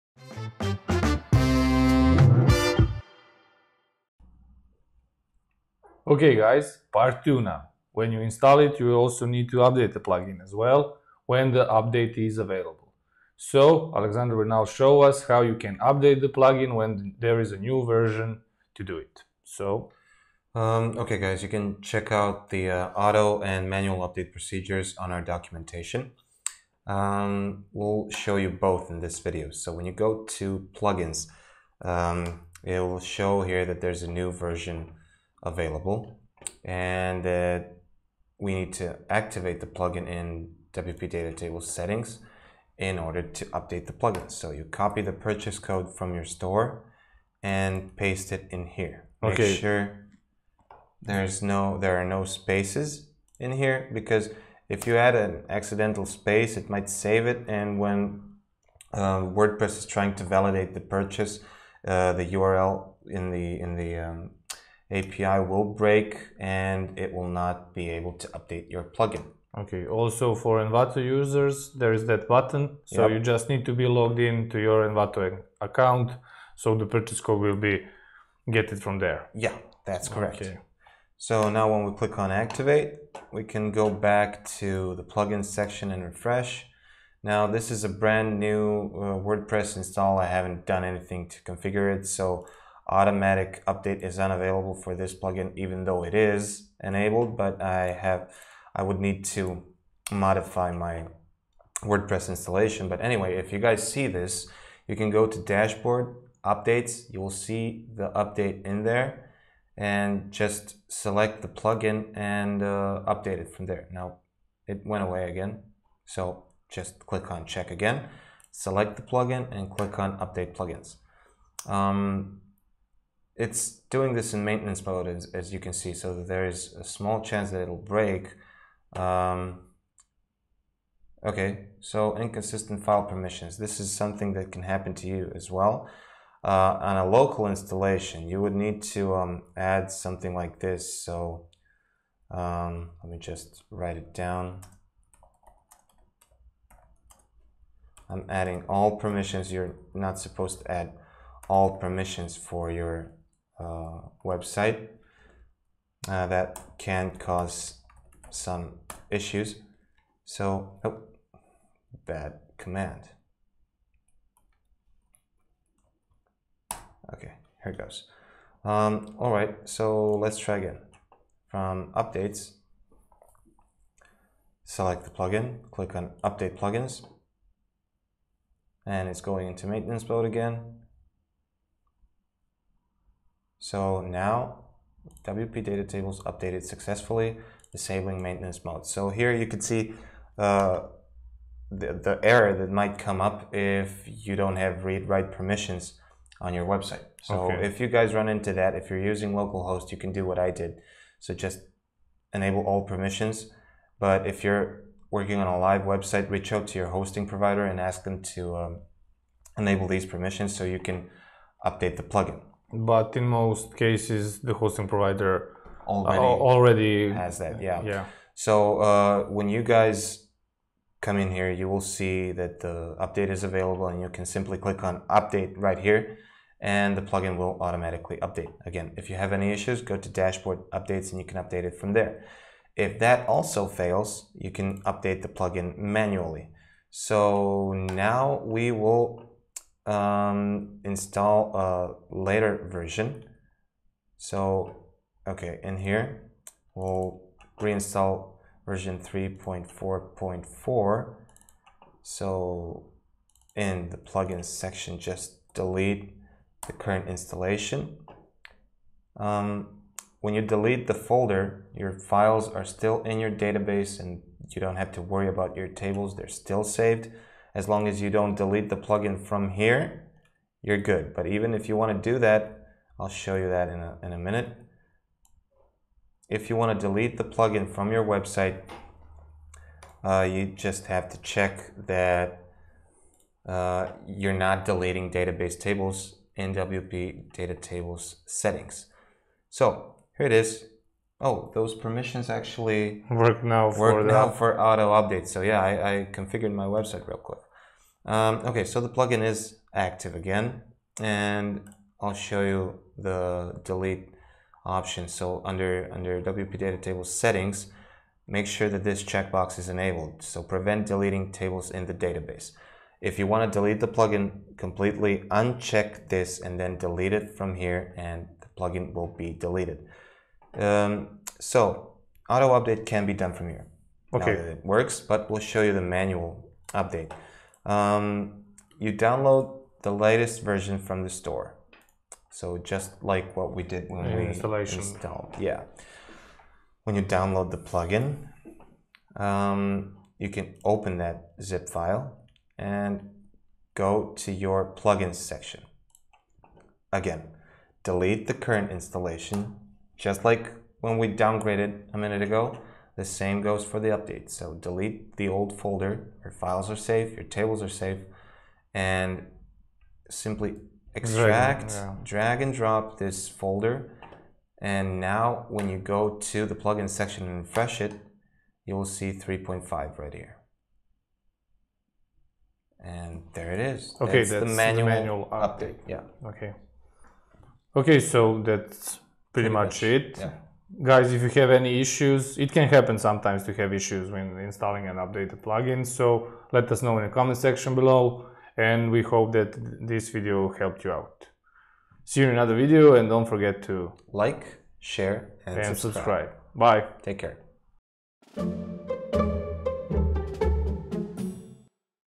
How to update wpDataTables plugin on WordPress website
In this video, we will show you how you can update wpDataTables, or any other plugin, on a WordPress website when the update is available.